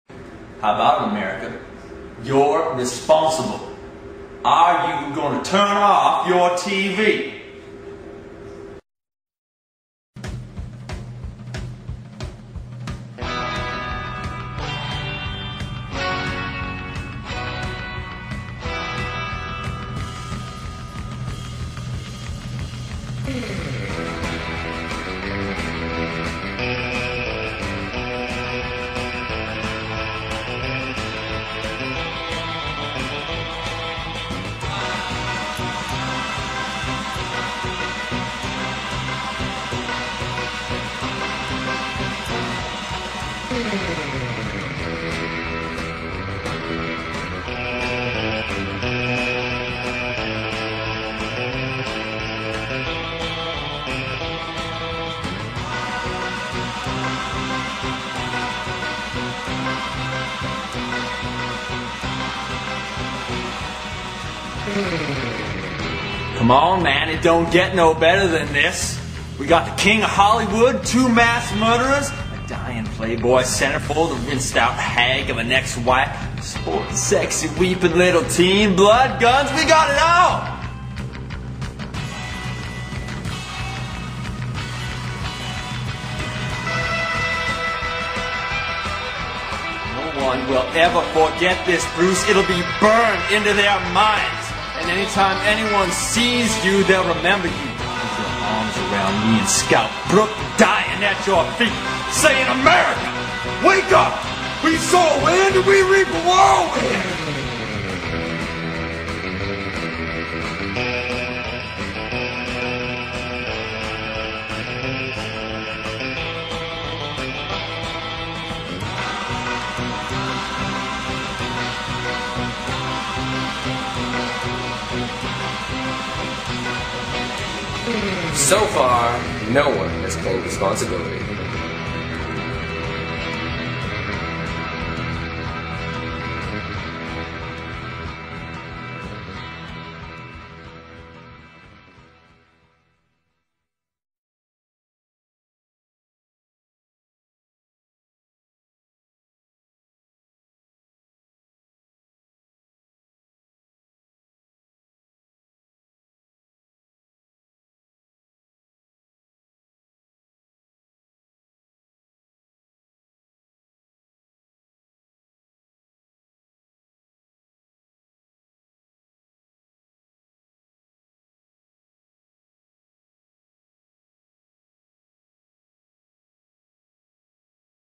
Popcorn Trailer
Mindful Phoenix Arts presents Ben Elton's play Popcorn, a fierce black comedy about violence and the media. The King of Hollywood, two mass murderers, a dying Playboy centerfold, a rinsed-out hag of an ex-wife, a spoilt, sexy weeping teen, blood, guns...we got it all! Shows in southern Taiwan December 2006.